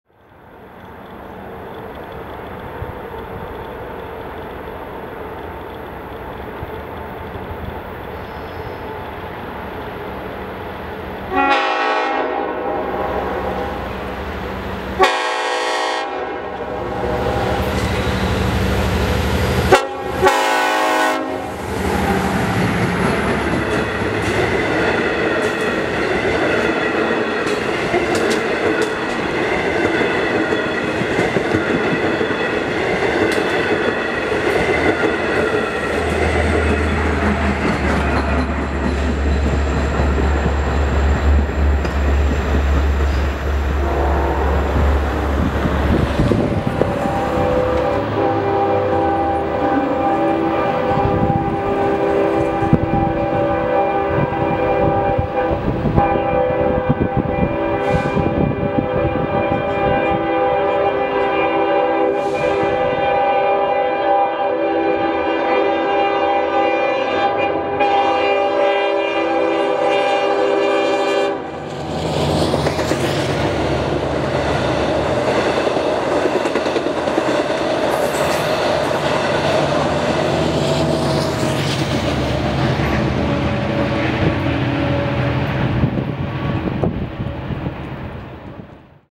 LIRR Mashed Potato Engineer Bakes Long Island with EMD DM30ACs
The "Spud" gives Louisiana's Fastest a run for their money with some five chime fanfare on a winter evening.